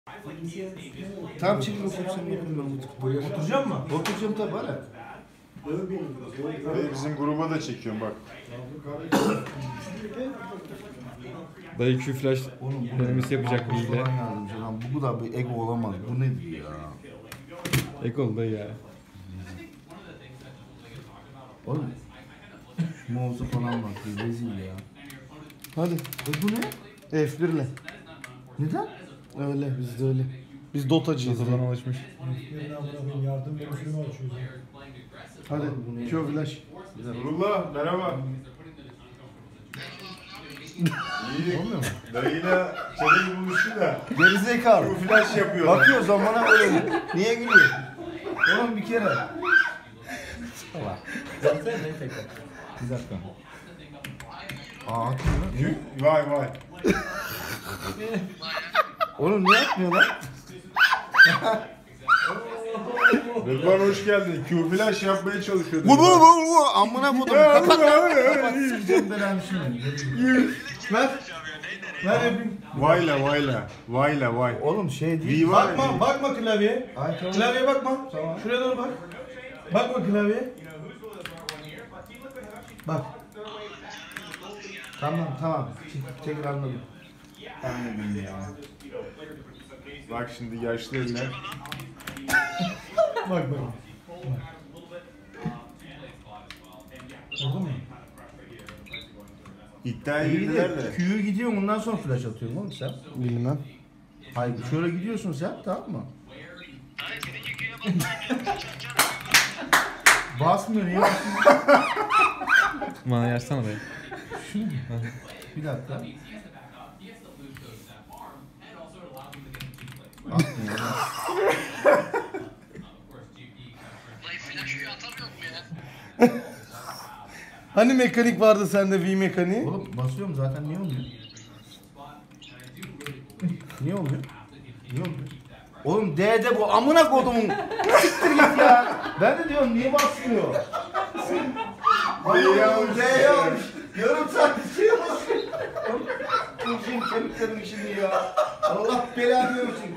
MALKOÇ ZARAKİ DAYIYI TROLLÜYOR (KÜFÜRLÜ)
Malkoç flashı d de kullandığı için zaraki dayı vi ile q flash yapamıyor 2 dakika boyunca denedikten sonra flashın d de olduğunu farkediyor.